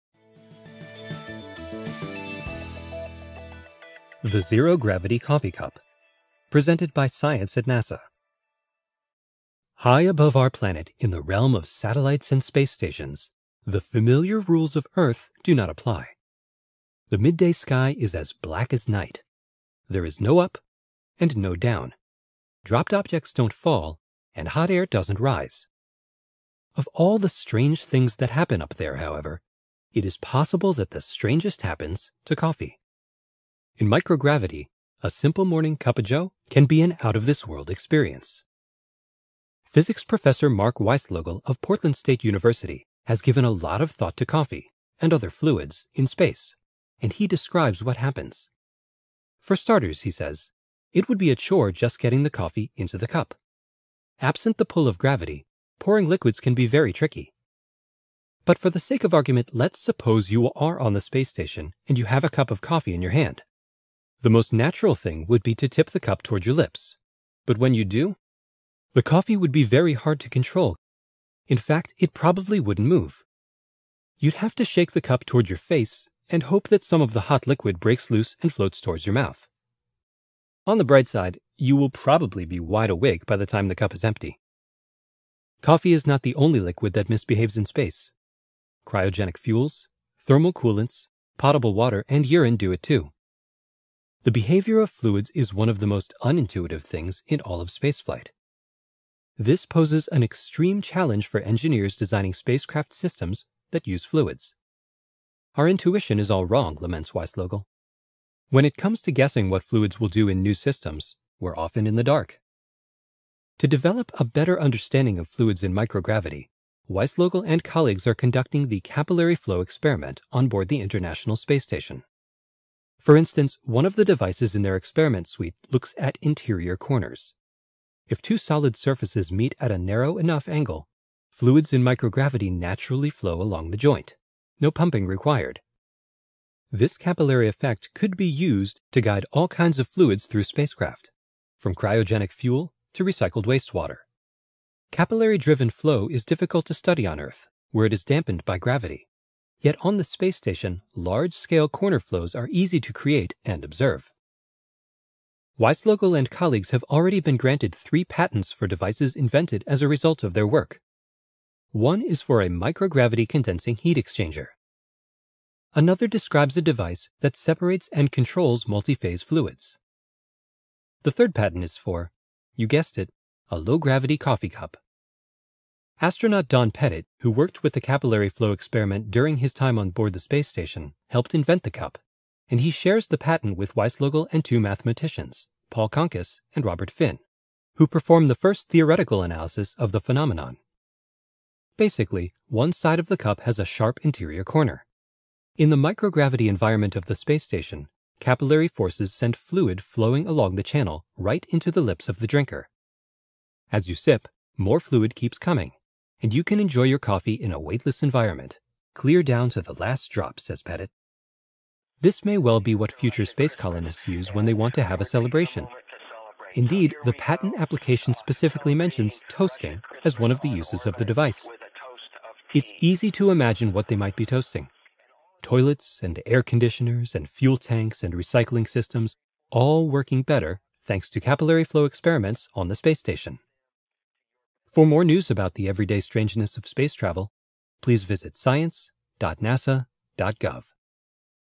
La taza de café gravedad cero
Beber café en el espacio es sorprendentemente difícil. Los físicos que investigan el extraño comportamiento de los líquidos a bordo de la Estación Espacial Internacional han inventado un taza de café cero-G para hacer por la mañana una "cuppa Joe" un poco más fácil de tragar.

El profesor de Física Marcos Weislogel de la Universidad Estatal de Portland ha dado en el espacio muchas vueltas al café (y a otros líquidos), y describe lo que sucede: "Para empezar", dice, "sería una gran tarea conseguir poner el café en la taza. Ante la ausencia de la fuerza de gravedad, verter líquidos puede ser muy difícil". "Pero, por el bien del argumento, vamos a suponer que usted está en la estación espacial y toma una taza de café en la mano. Lo más natural sería inclinar la taza hacia sus labios, pero cuando lo hace... "El café sería muy difícil de controlar", continúa. "De hecho, probablemente no saldrá de la taza. Habría que agitar la taza hacia la cara con la esperanza de que parte del líquido caliente se desprenda y flote hacia la boca".

Saber más:
Taza de café de gravedad cero para el espacio